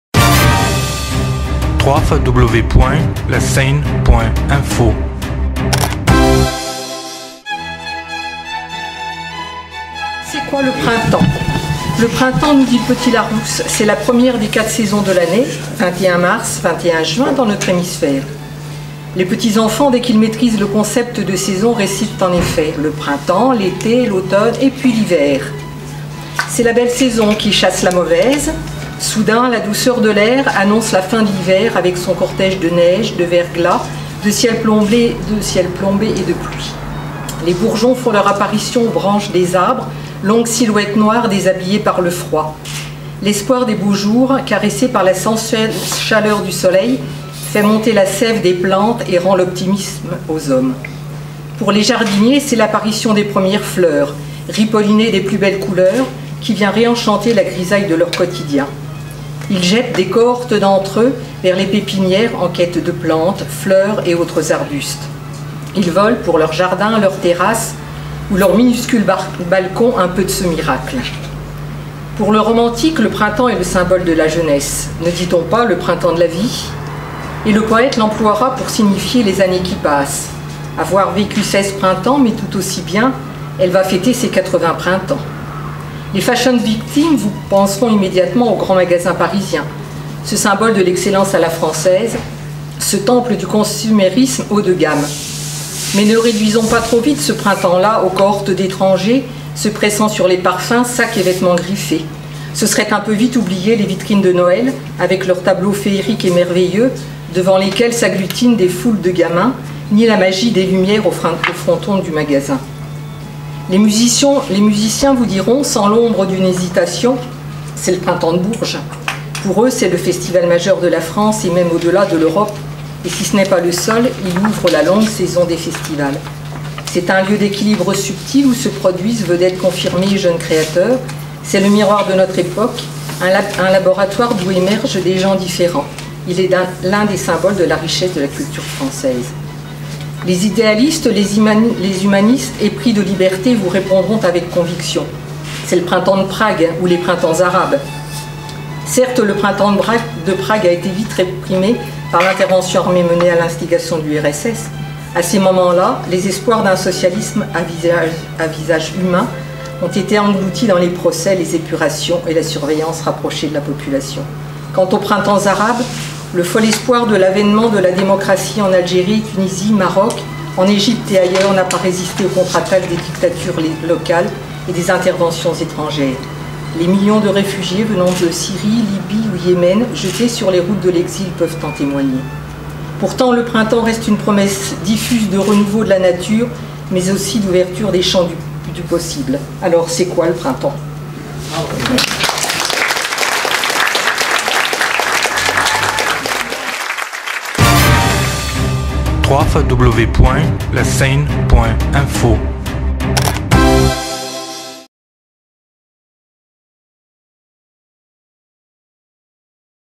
Marie Janick MAYBEL - C’est quoi le Printemps
Marie Janick MAYBEL - C’est quoi le Printemps - L'association Le Cri de La Seyne Info a organisé le concours de nouvelles et de poésie 2017 ...